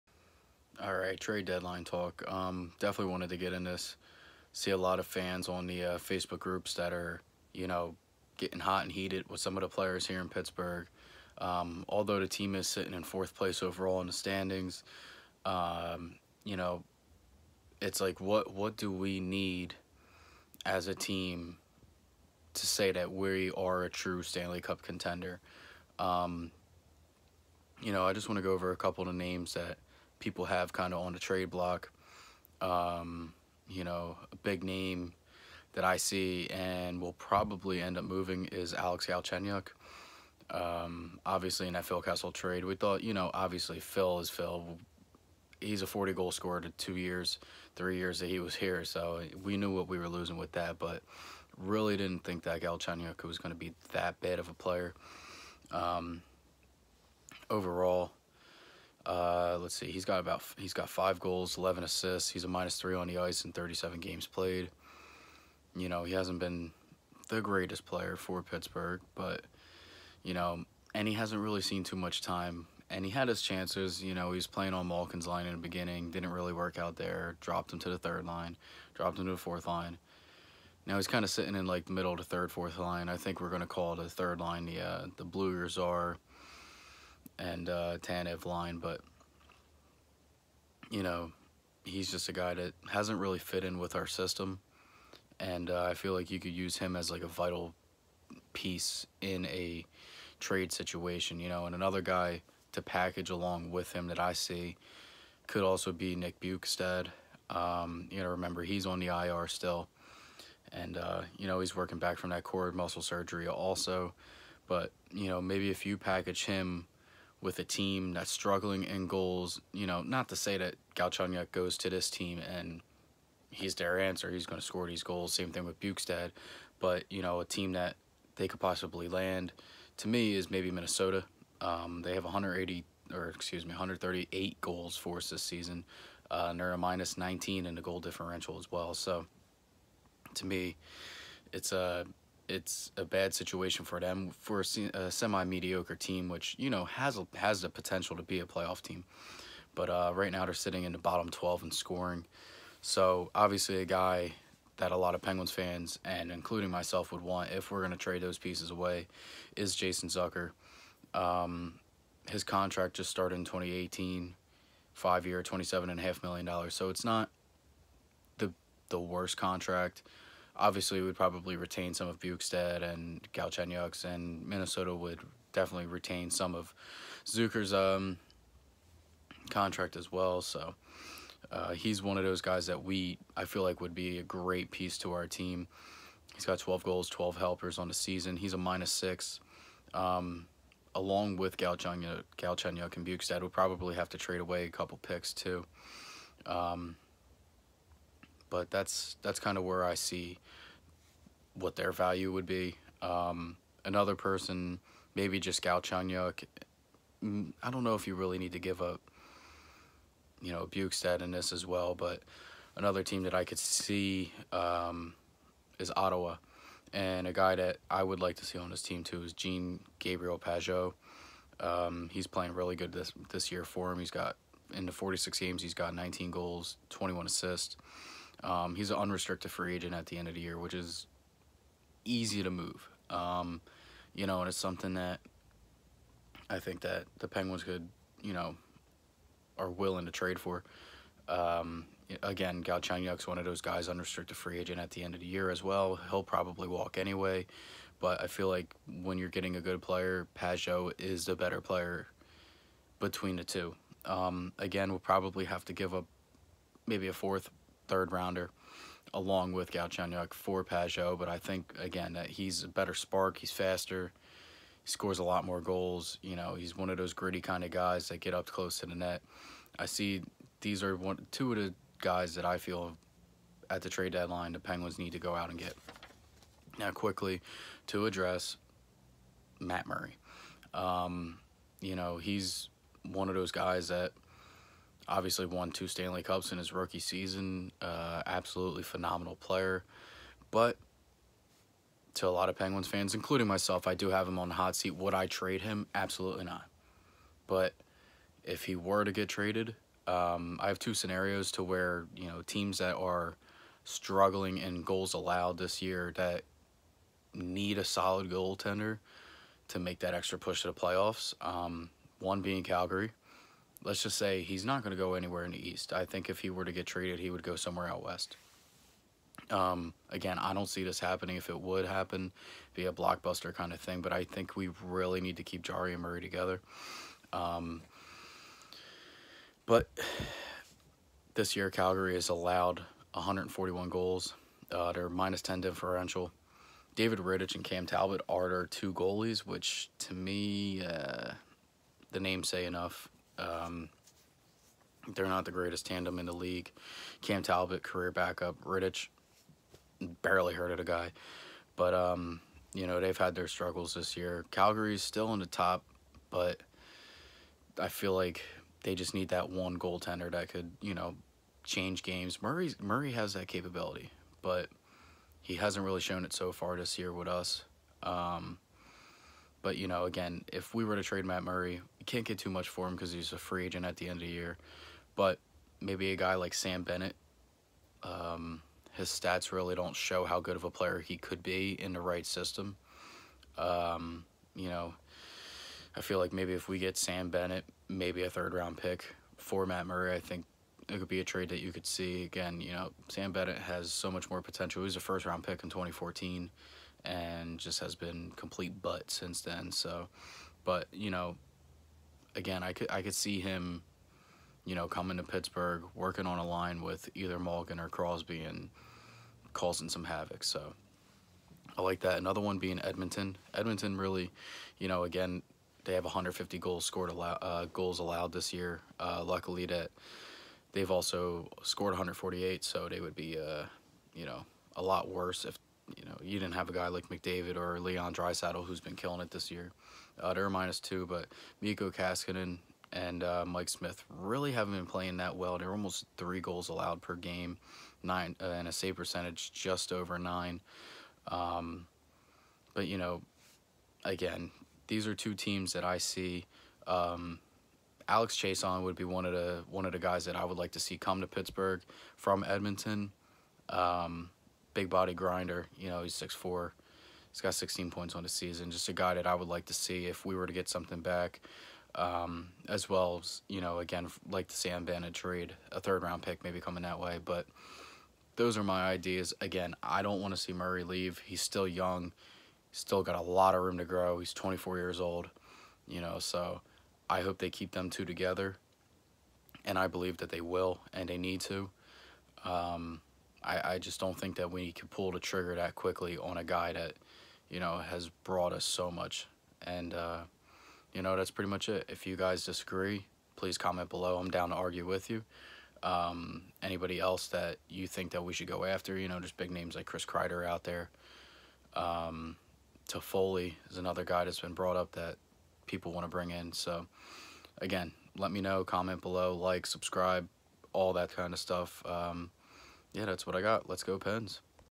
Pittsburgh Penguins Trade Rumor Talk
Trade rumor talk. Players that people are saying they want, possible scenarios that could work. I believe Pittsburgh keeps Murray, maybe loses him in the expansion draft.